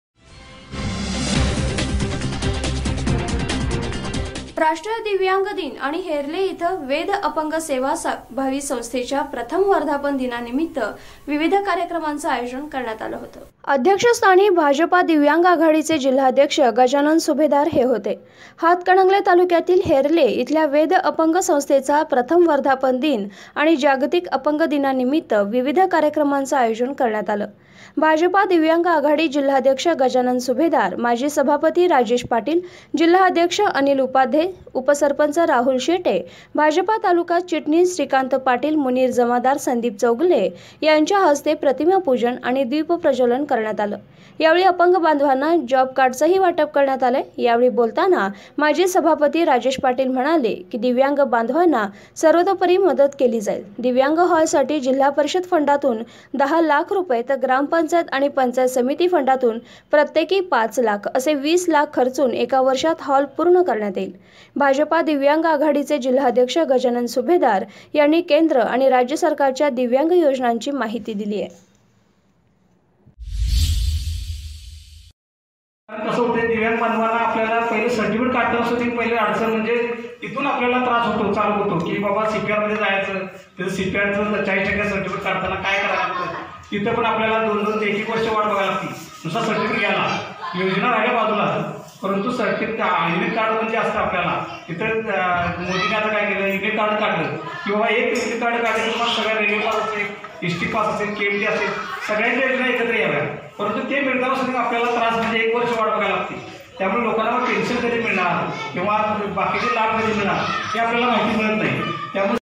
HERLE | हेरले येथील वेद अपंग सेवा भावी संस्थेच्या प्रथम वर्धापन दिनानिमित्त विविध कार्यक्रम
हेरले येथील वेद अपंग सेवा भावी संस्थेच्या प्रथम वर्धापन दिनानिमित्त विविध कार्यक्रम.@